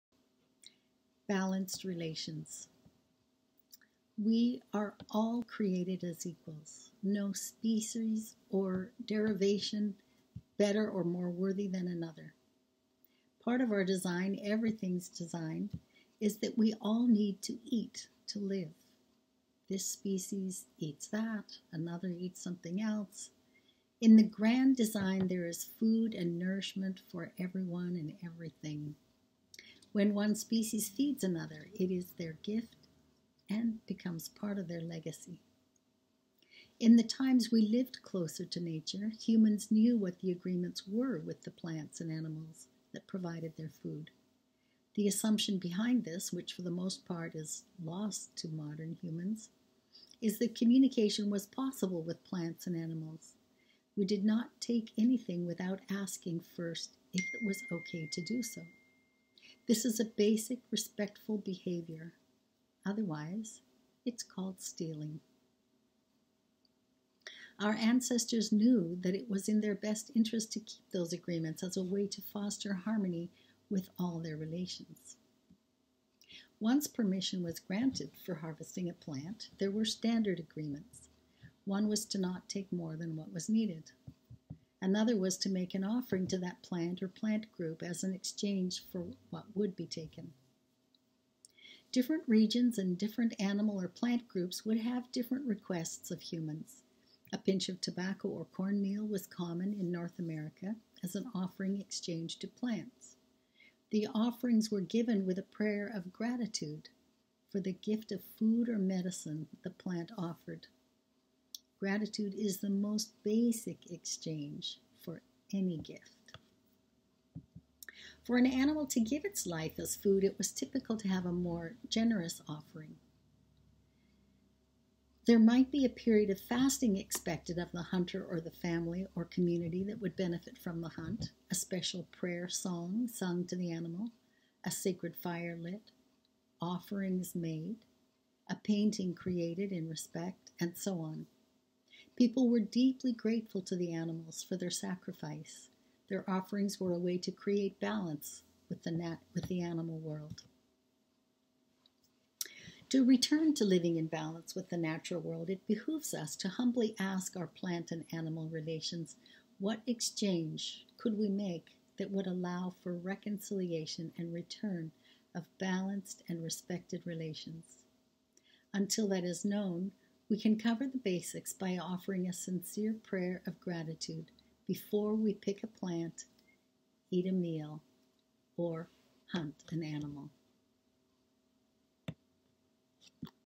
Movie on 2020 04 07 at 3 08 PM
Harmonious Relationships
One important way we can return to living in harmony with the world around us.